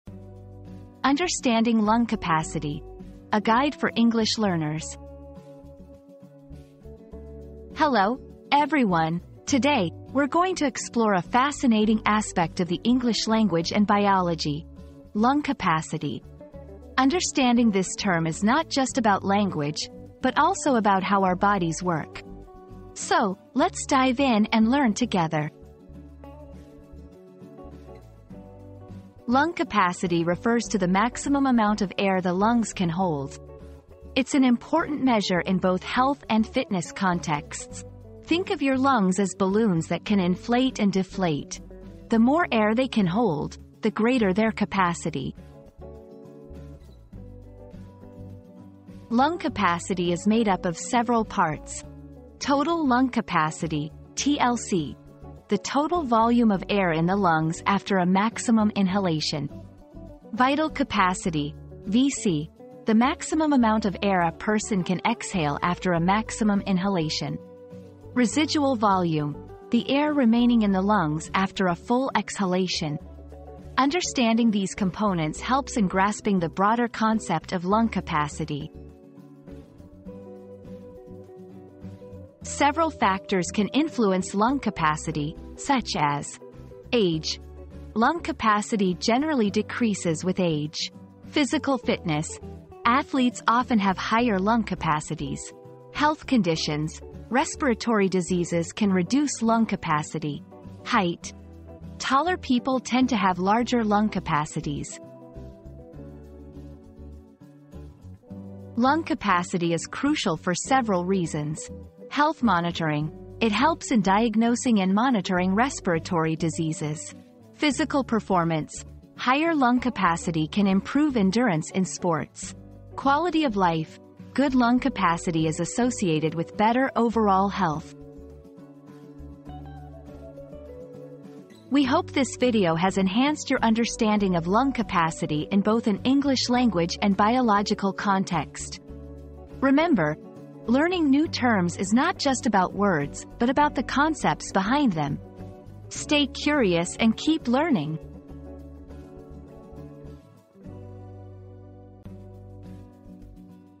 Understanding Lung Capacity: A Guide for English Learners
Unlocking Lung Capacity: A Guide for English Learners • Discover the secrets to expanding your lung capacity in this comprehensive guide designed specifically for English learners. Learn essential techniques and exercises to improve your breathing and enhance your overall well-being. Take your English fluency to new heights while unlocking the power of your lungs. Watch now!

00:00 • Introduction - Understanding Lung Capacity: A Guide for English Learners
00:28 • What is Lung Capacity?
00:48 • The Components of Lung Capacity
01:21 • Factors Affecting Lung Capacity
01:48 • Why is Lung Capacity Important?